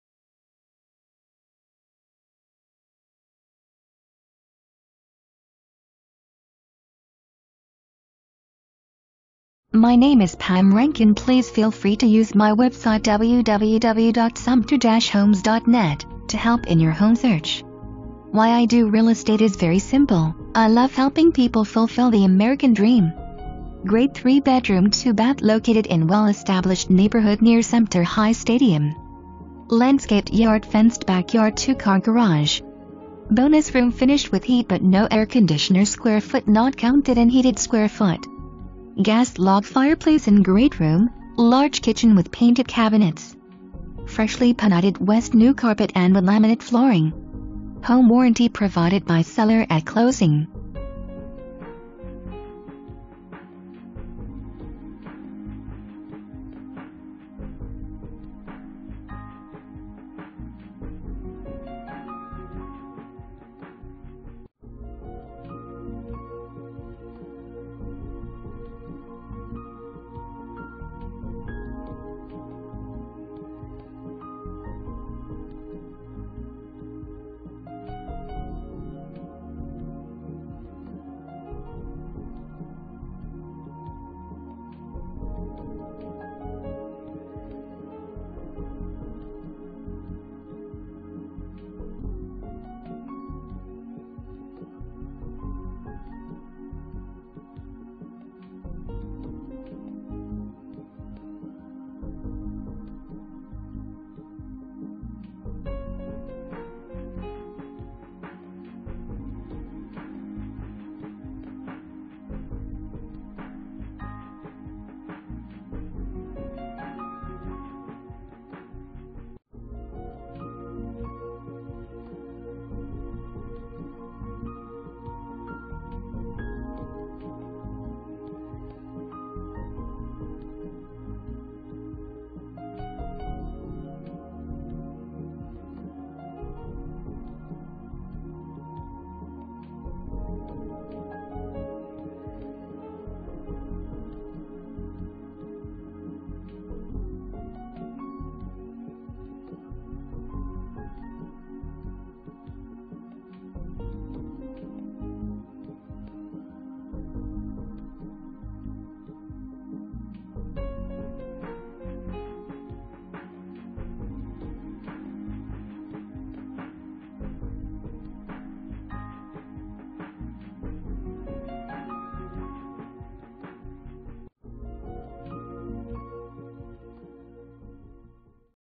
1052 Saltwood Drive
Great 3 bedroom 2 bath located in well established neighborhood near Sumter High Stadium. Landscaped yard fenced back yard 2 car garage. Bonus room finished with heat but no air conditioner sq ft not counted in heated sq ft. Gas log fireplace in great room, large kitchen with painted cabinets. Freshly panited w/new carpet and wood Laminate flooring. Home Warranty provided by seller at closing

Presented By:
Pam Rankin, Advantage Realty Group, Inc.